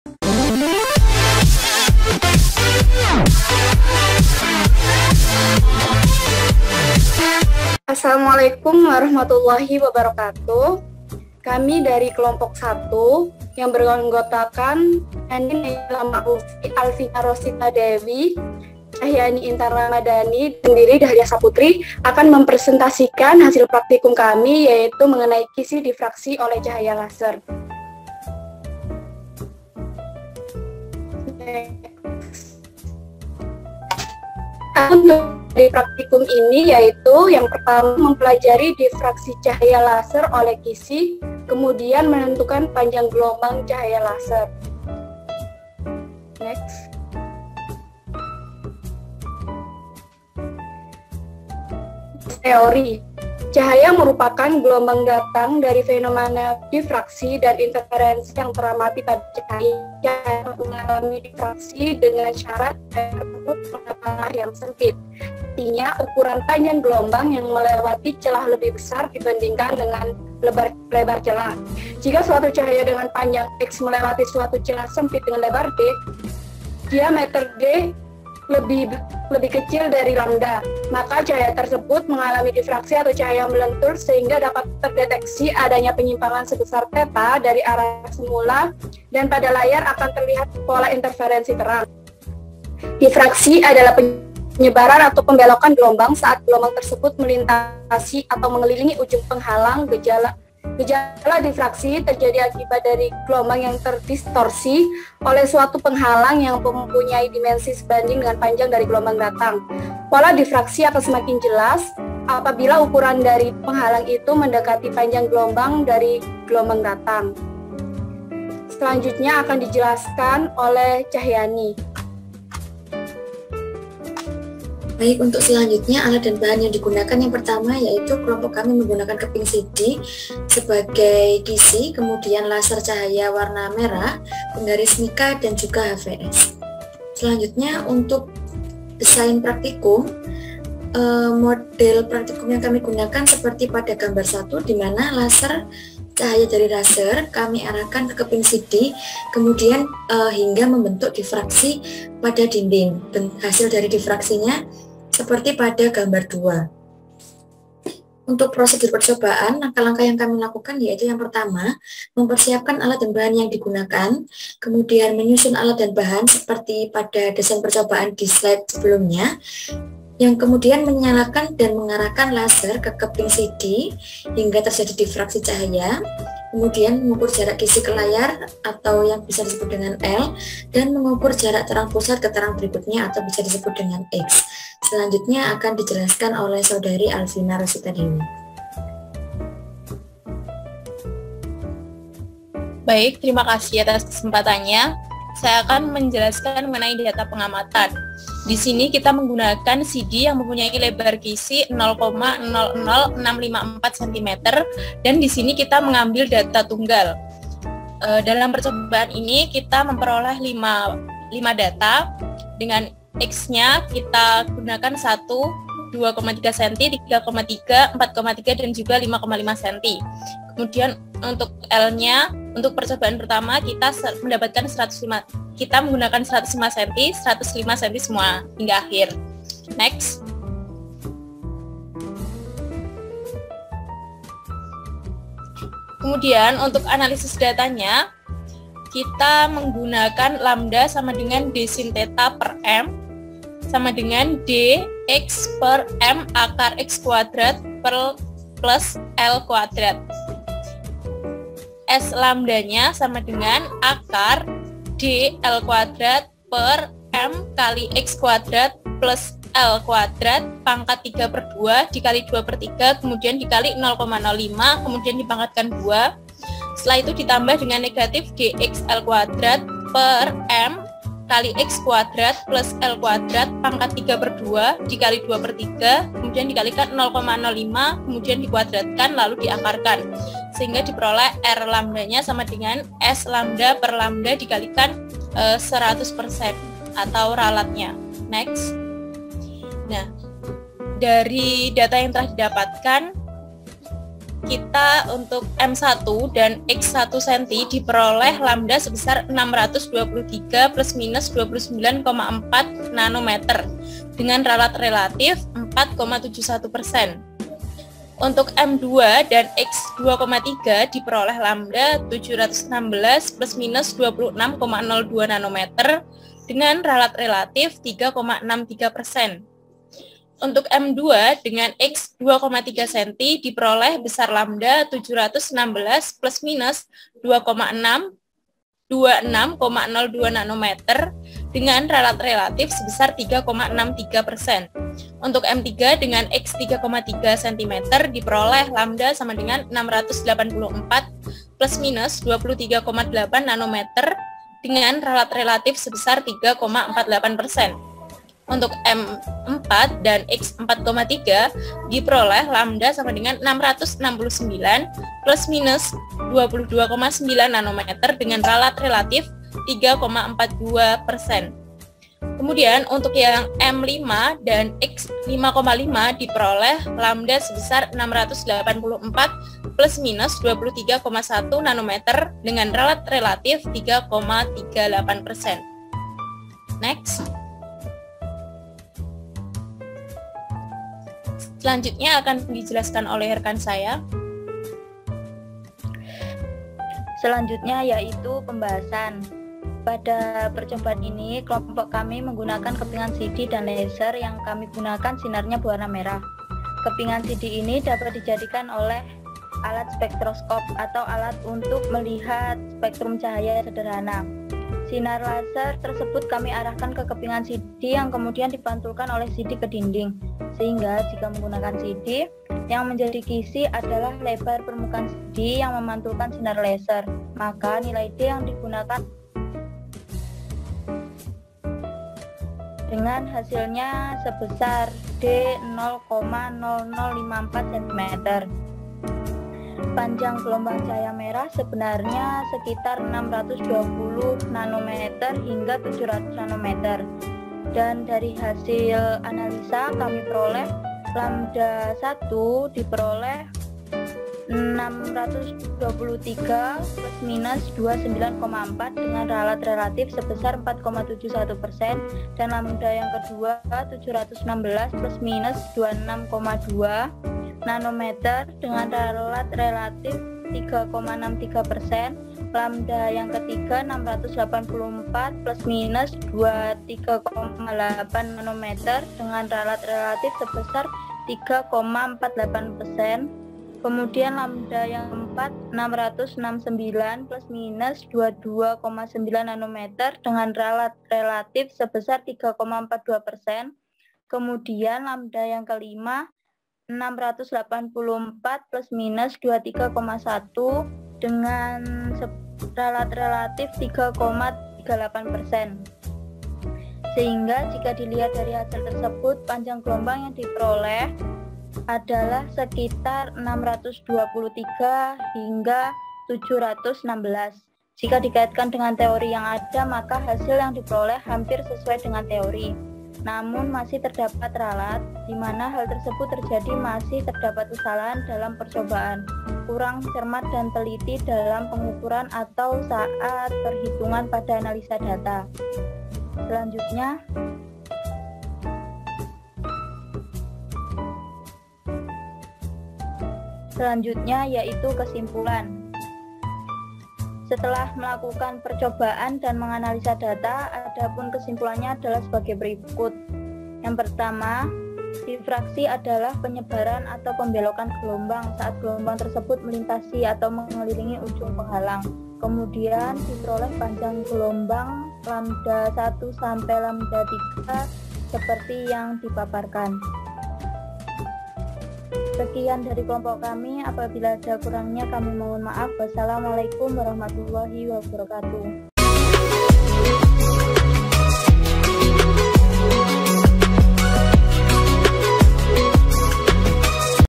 Praktikum Optika-Kisi Difraksi oleh Cahaya Laser
Praktikum Optika
Kisi Difraksi oleh Cahaya Laser
Kelompok 1 Offering B S1 Pendidikan Fisika
Universitas Negeri Malang